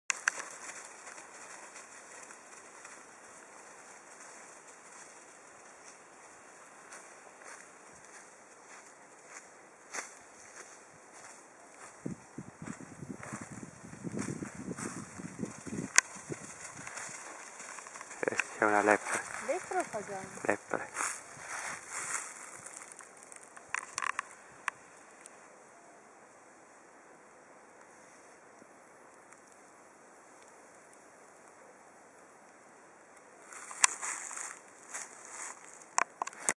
Elfin welsh springer spaniel and Plume Field spaniel
Searching  action on hare scent in wood